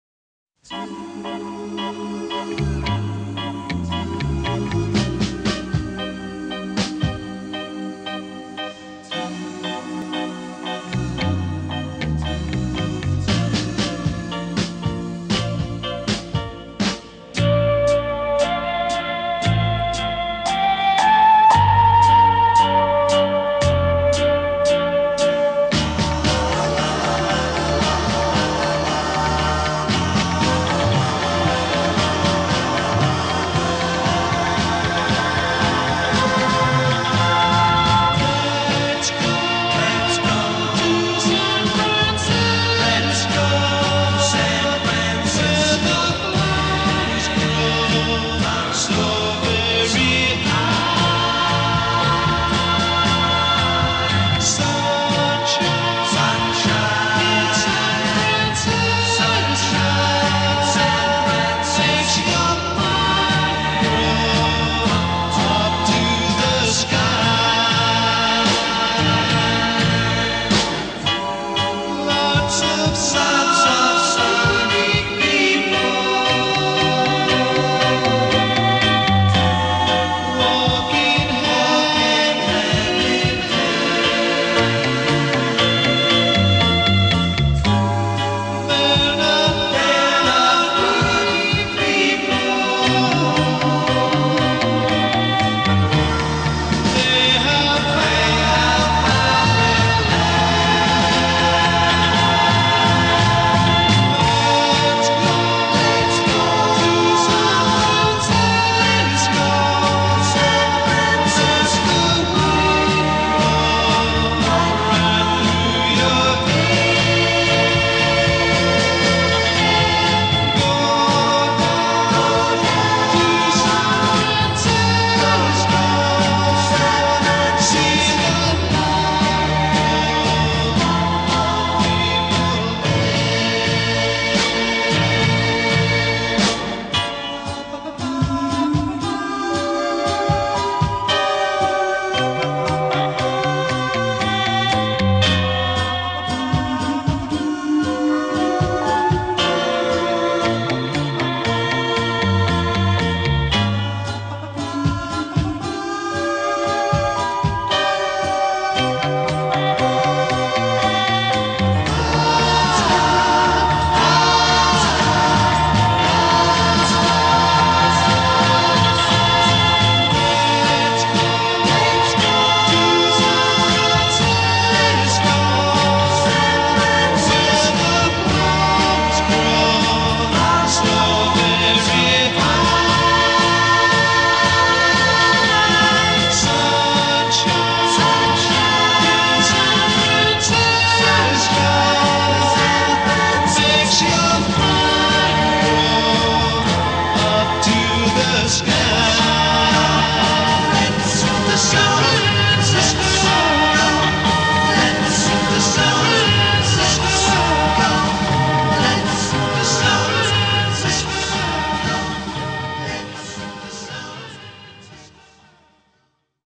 Let's Go To San Francisco, Ouer Roadtrip 7.2010
This is the begin and the end of ouer roadtrip in California, Nevada and Arizona. Marko and Honza